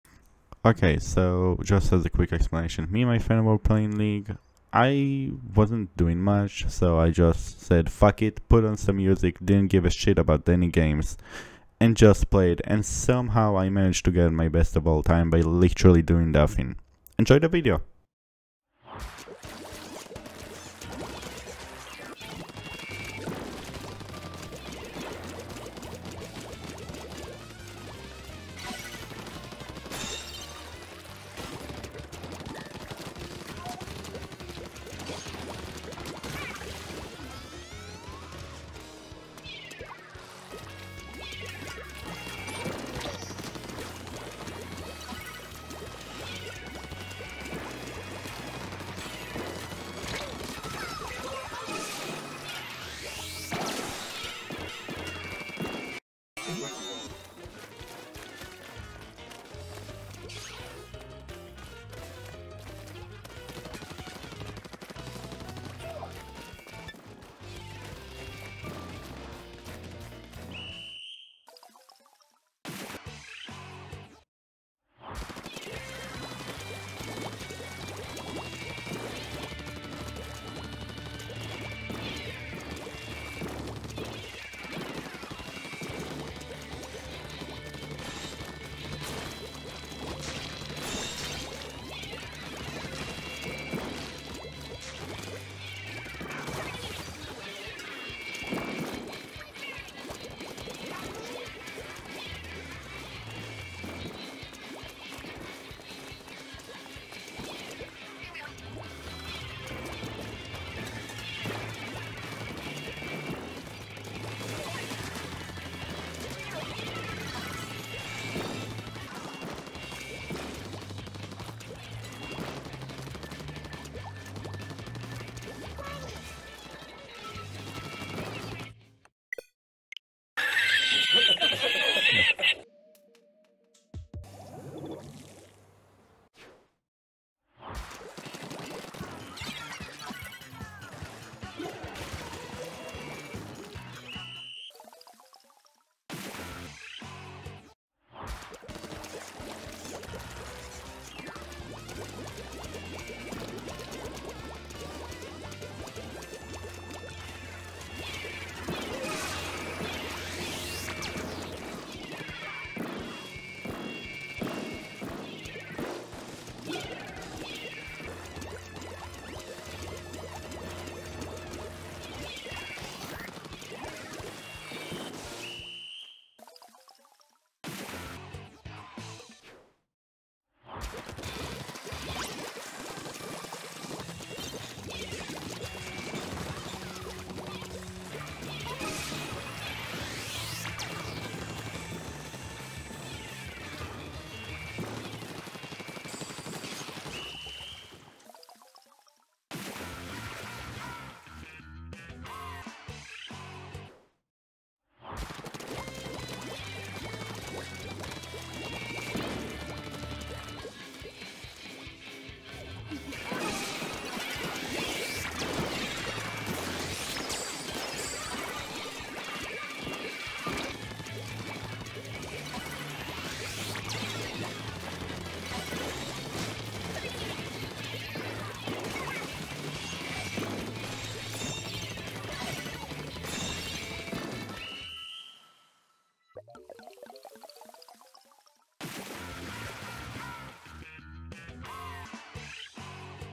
Getting My Highest League Power By Trolling
i didnt do shit and got carried by my teammate but still managed to do good enough to get a new all time high